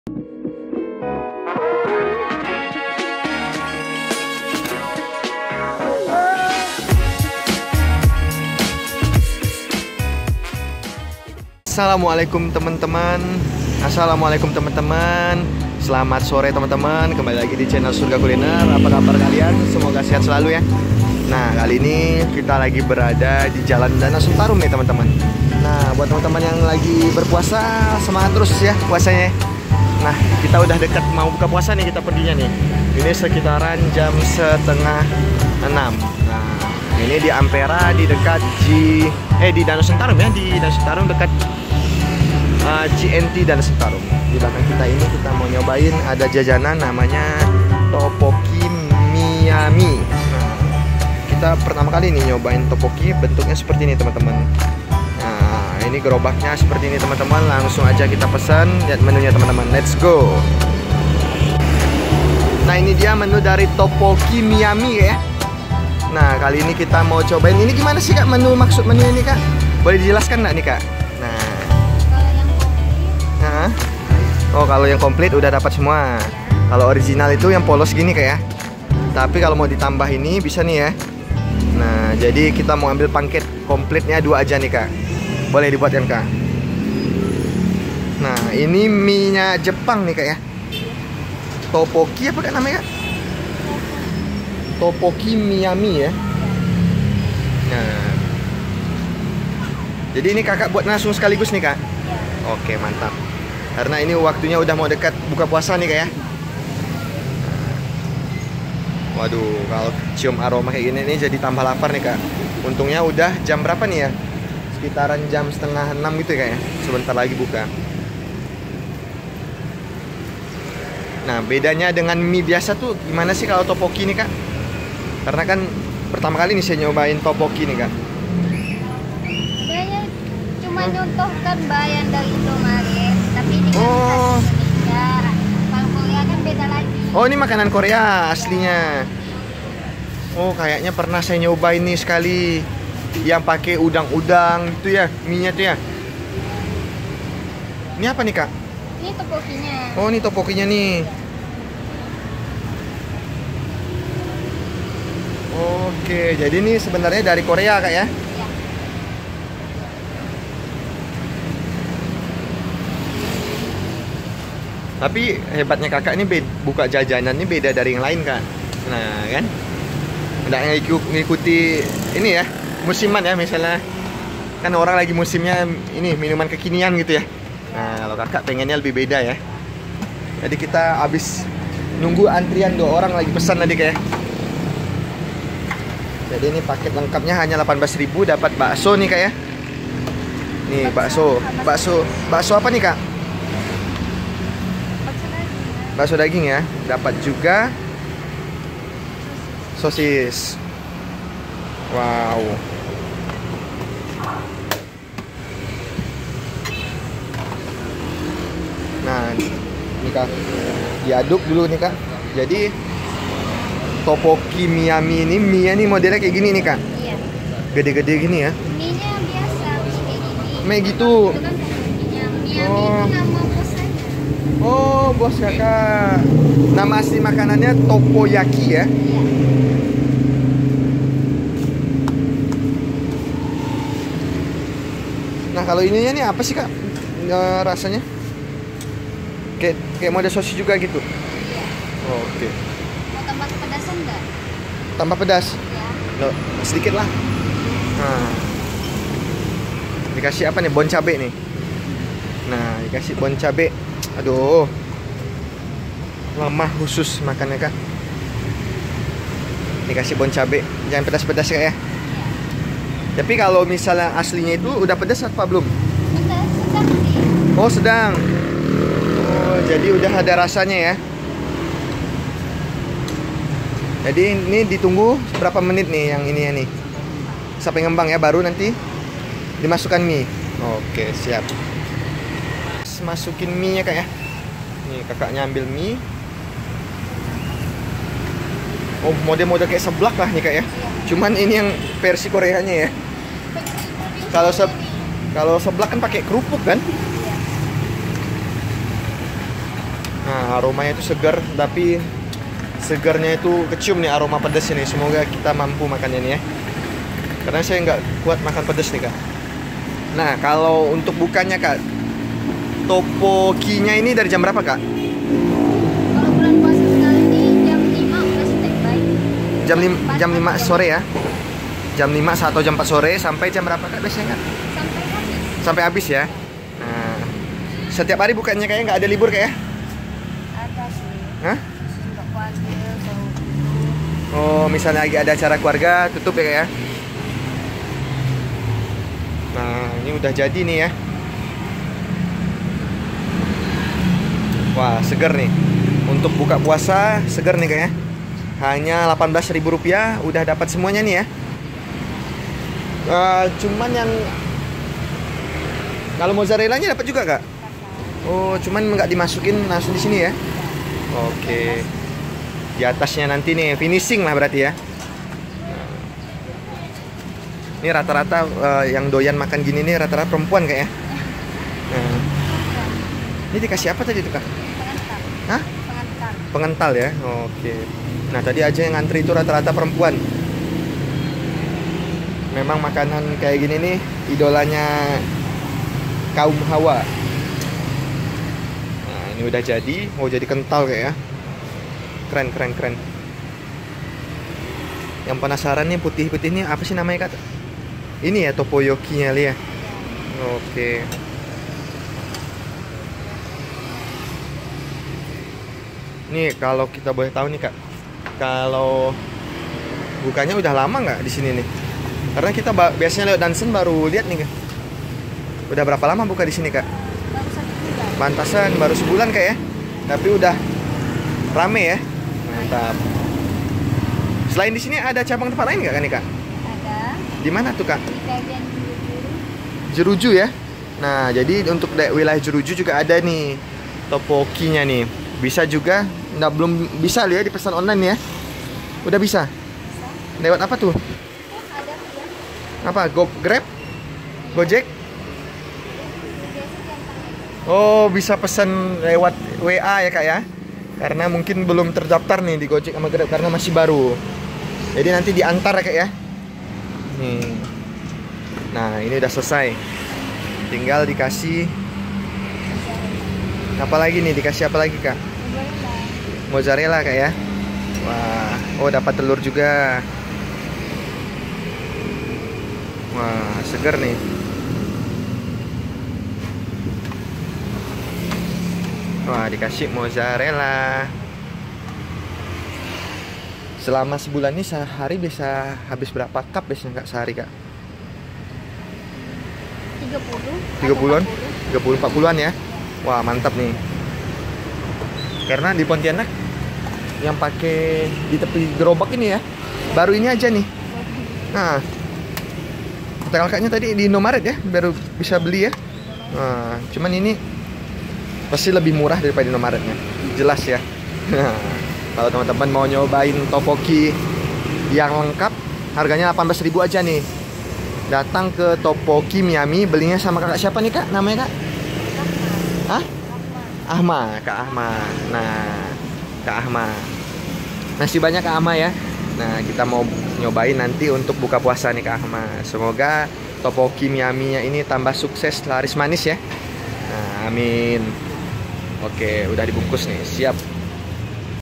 BIKIN CEWEK ANTRI! MODAL GEROBAK IMUT BISA SAINGI RESTORAN MEWAH | INDONESIA STREET FOOD
Lokasi: Jl. Danau Senatrum, Pontianak

Untuk Kalian Pecinta Jajanan Kami Memberikan Informasi Kuliner Kuliner Terbaik